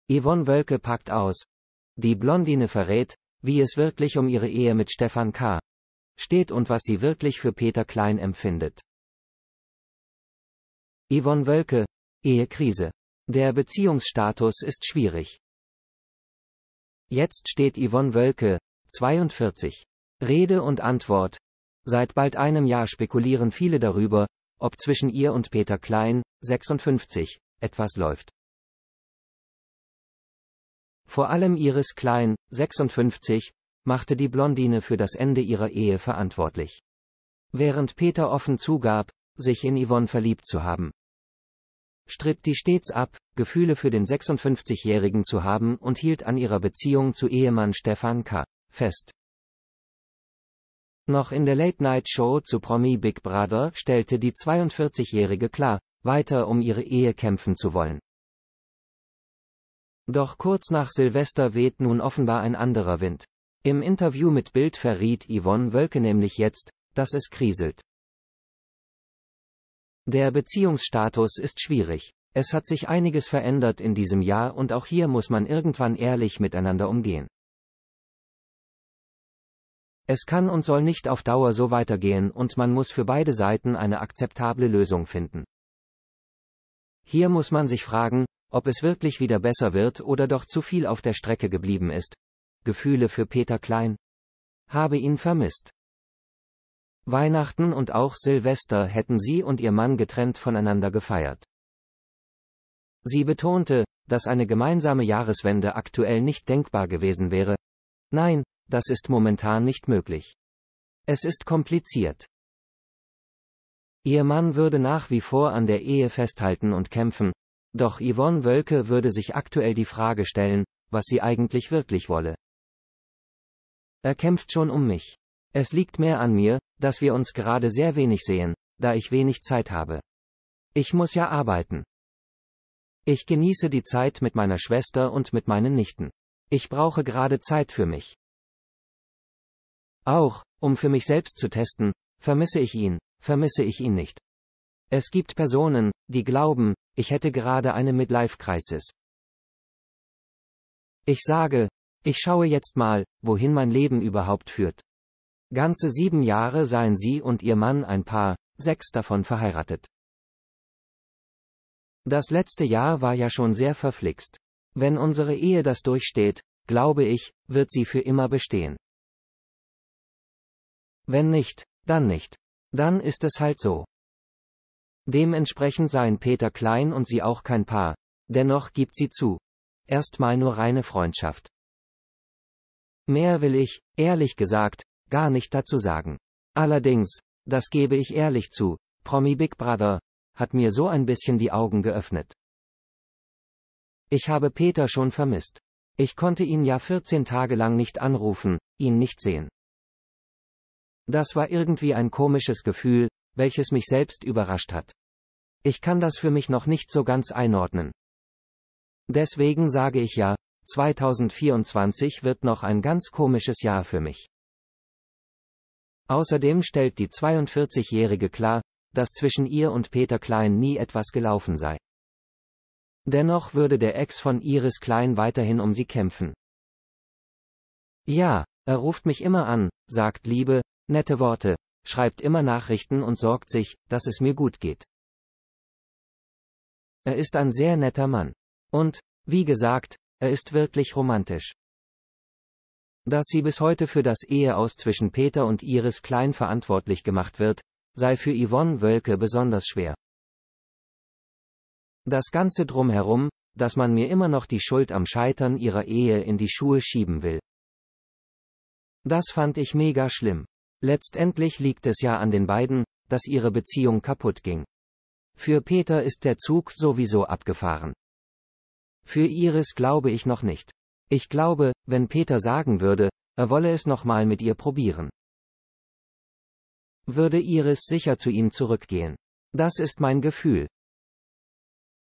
Yvonne Woelke Habe Peter vermisst Ehe Krise mit ihrem Mann Stephan
Yvonne Woelke packt aus: Die Blondine verrät, wie es wirklich um ihre Ehe mit Stephan K. steht und was sie wirklich für Peter Klein empfindet.